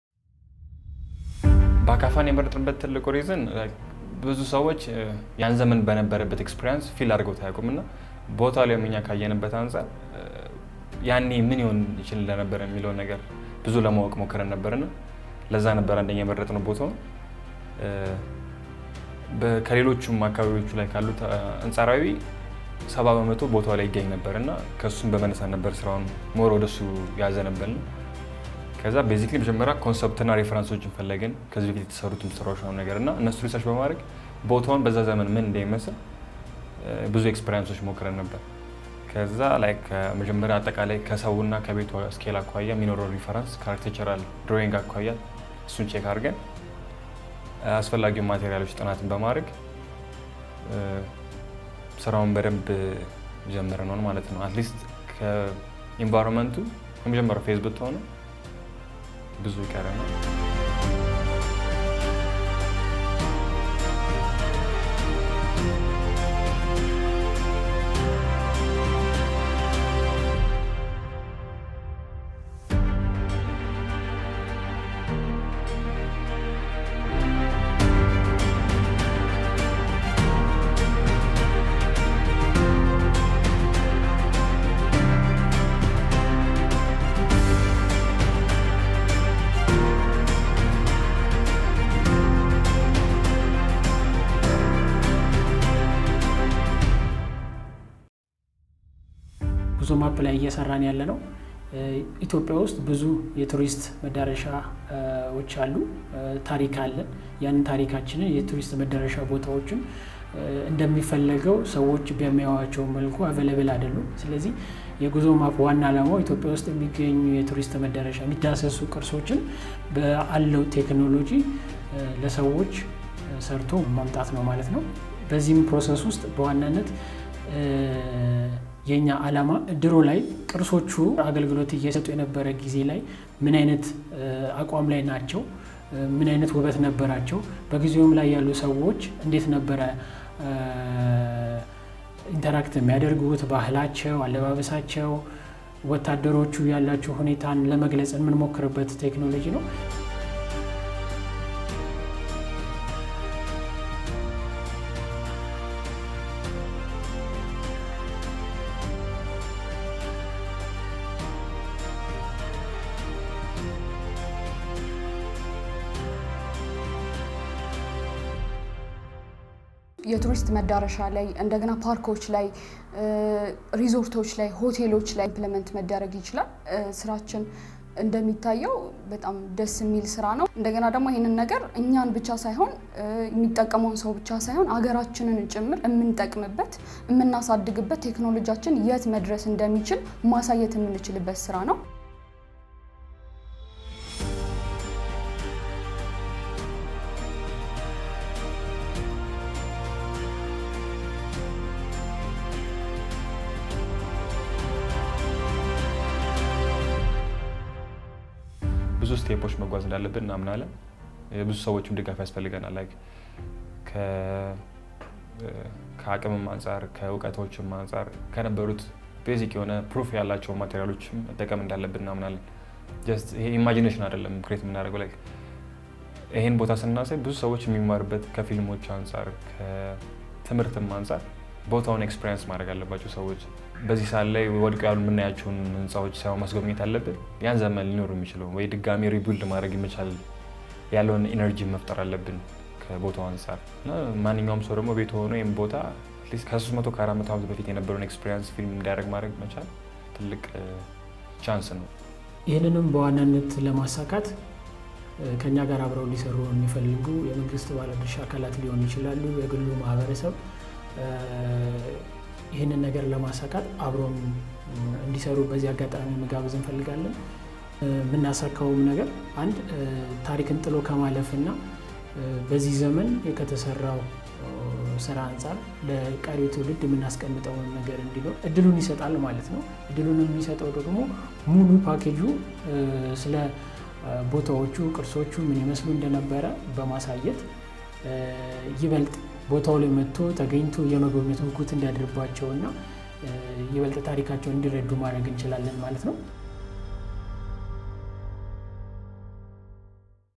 Short Documentary - The making of 'Bakaffa's Palace' VR - Powered by Guzomap
A short documentary about first 3D immersive experience of 'Bakaffa's Palace' that is located at Fasil Ghebi, Gondar, Ethiopia. The video walk you through how our platform Guzomap enable the tourism sector more presentable in the digital world.